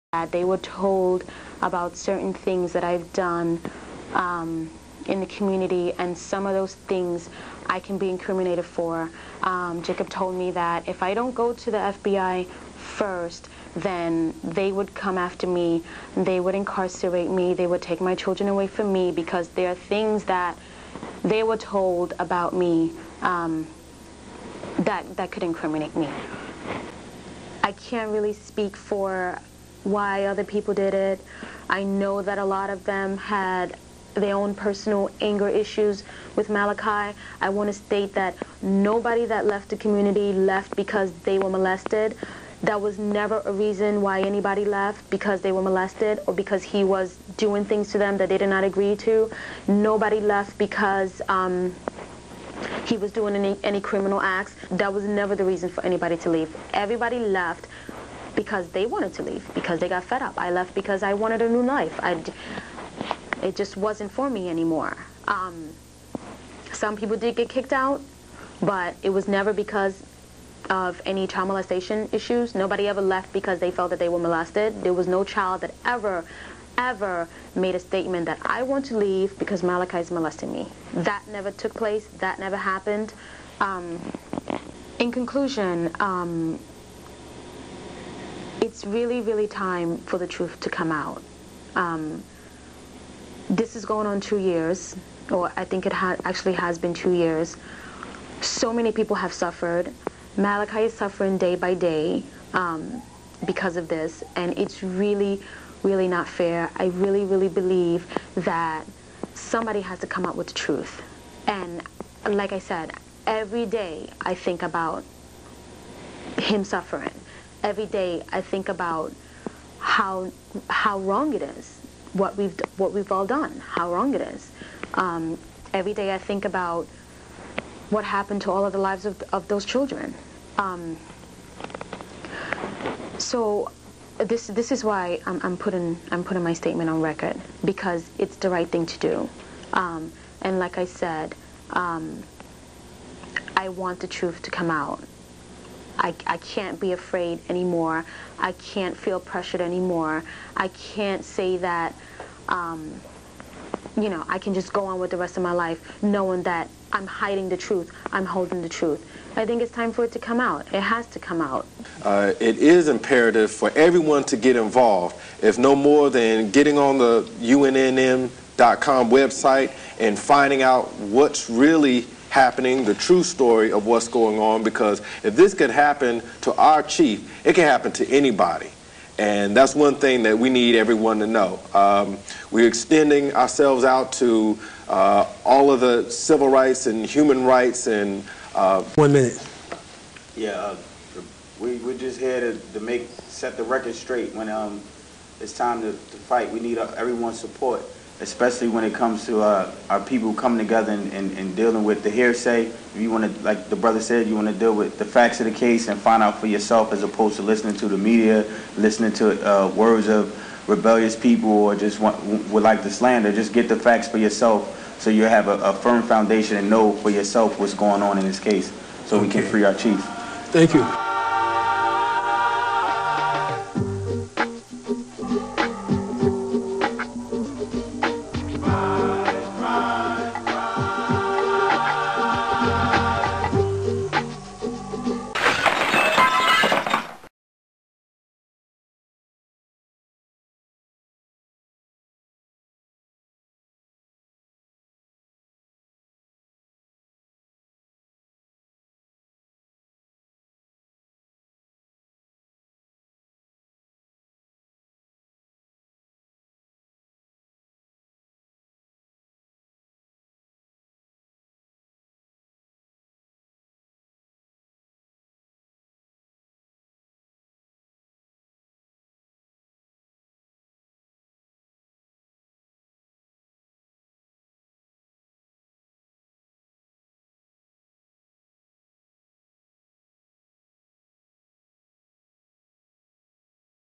Part 6 - Nuwaubians Conspiracy
Part 1 - Nuwaubians Conspiracy
Members of the Yamassee Native Americans and their attorney Adrian Patrick, speaks about suppress evidence regarding Dr. Malachi Z. York. This evidence proves beyond a shadow of a doubt that its a CONSPIRACY.